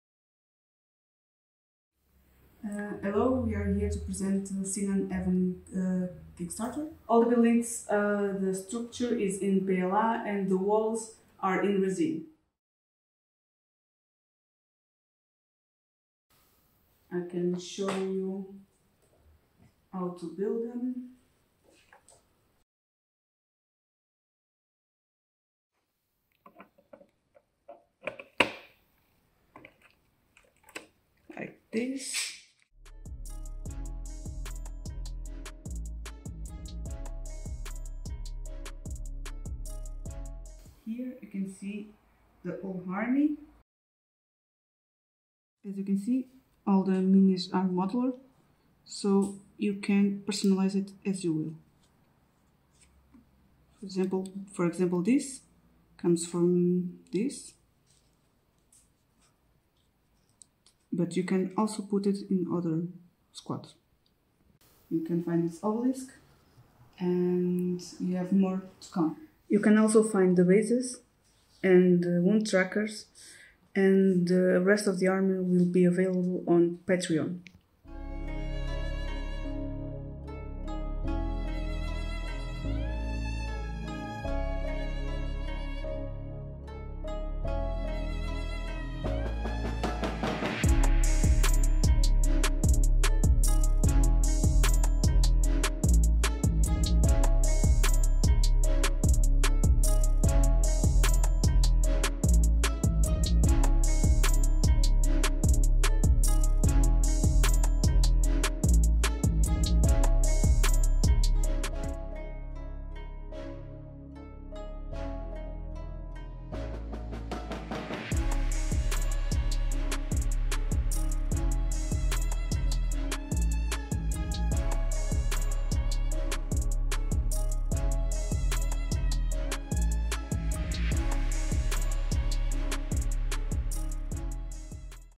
Cinan Haven Part 2, Modular buildings & Miniatures army
x9 Squads of modular miniatures
x30 Solid structure 3D printable in PLA.

x30 Shapes for structures in PLA.
x75 Double sided walls printable either in Resin or PLA.